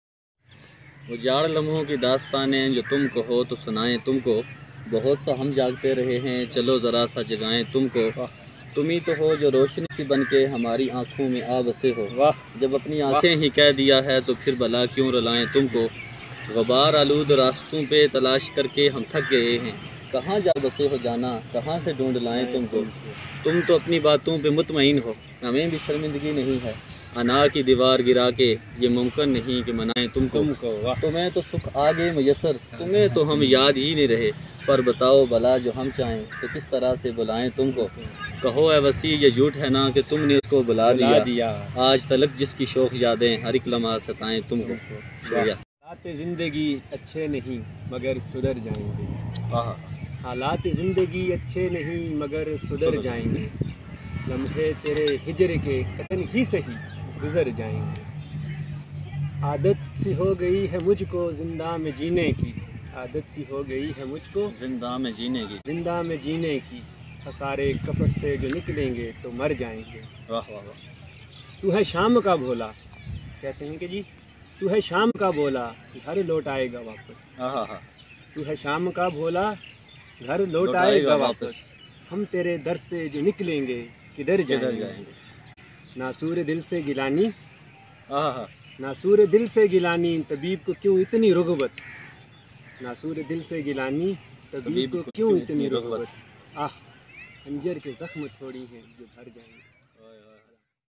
Ujaar lamhon ki dastanain jo tum kaho to | Tauseef Raja poetry |Noman Gillani poetry | POLICE WALA
اجاڑ لمحوں کی داستانیں جو تم کہو تو سنائیں تم کو،
بہت سا ہم جاگتے رہے ہیں، چلو زرا سا جگاہیں تم کو..